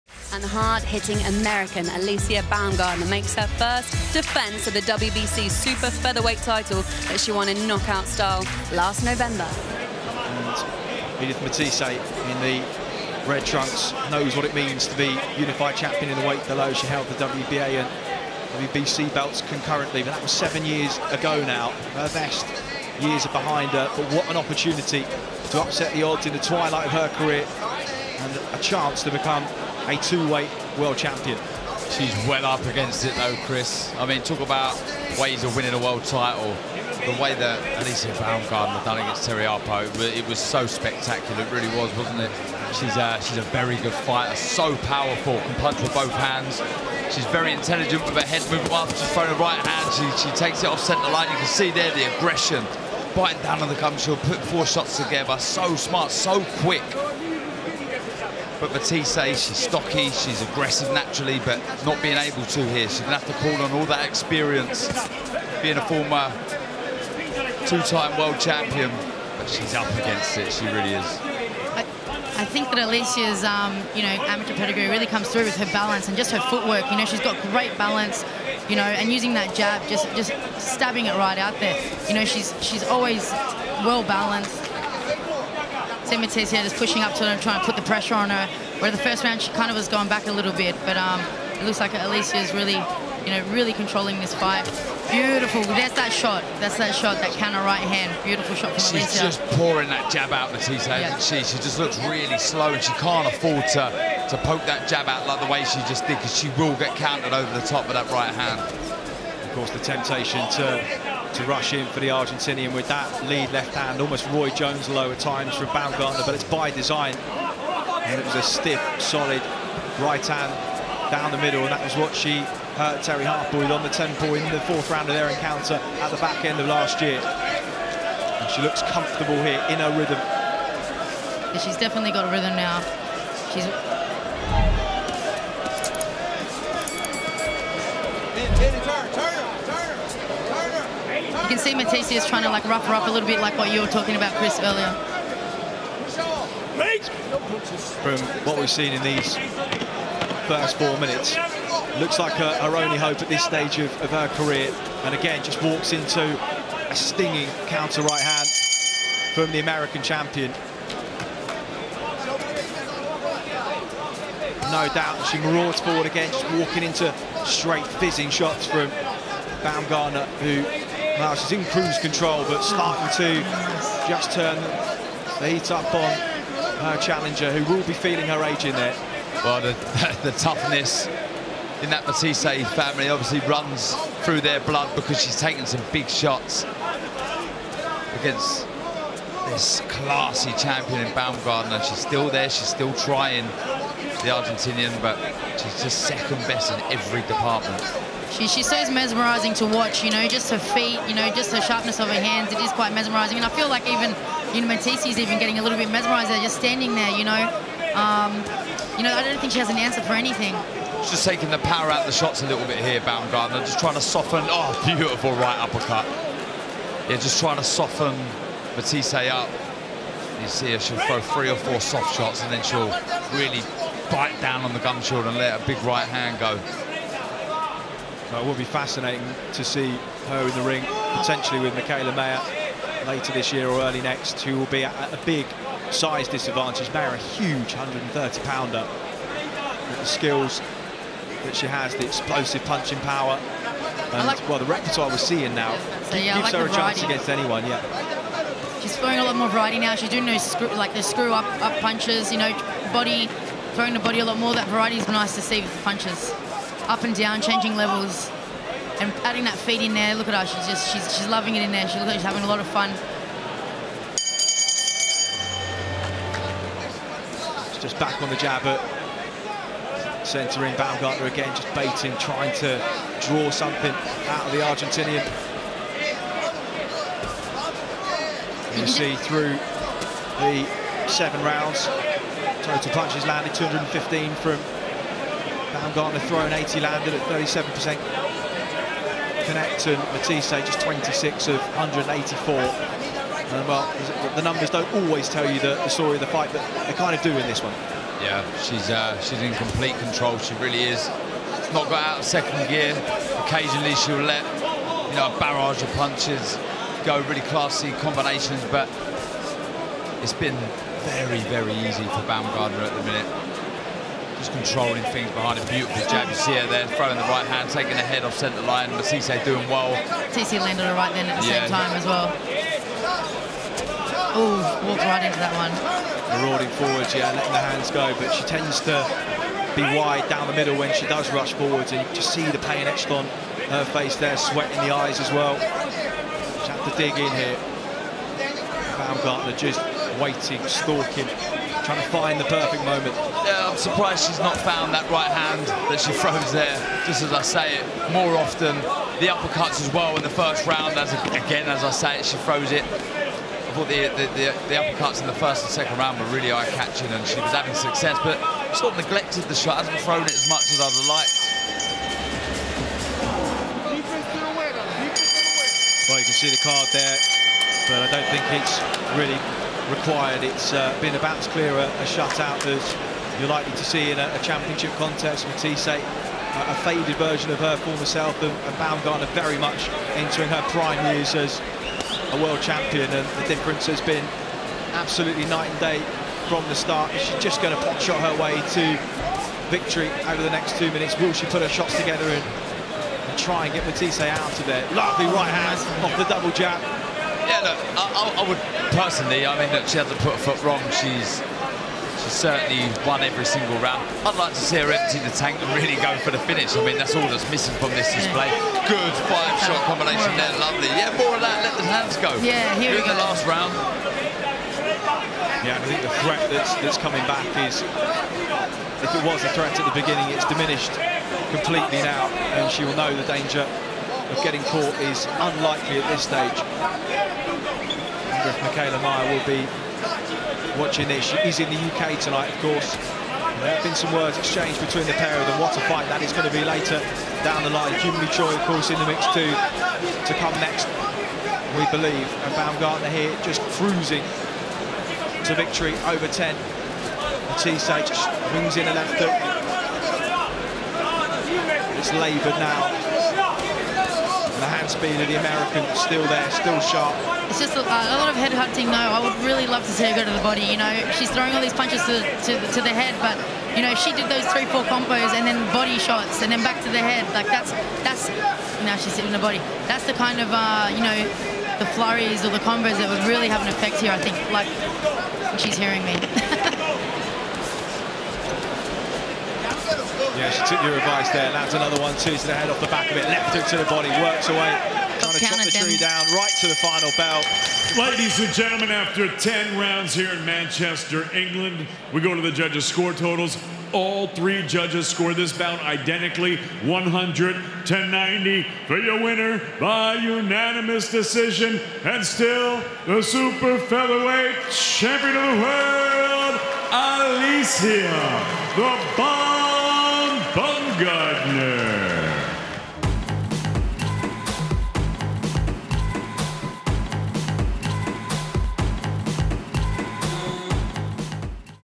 FIGHT HIGHLIGHTS | Alycia Baumgardner vs. Edith Soledad Matthysse
April 16th, 2022 -- Alycia Baumgardner vs. Edith Soledad Matthysse from Manchester.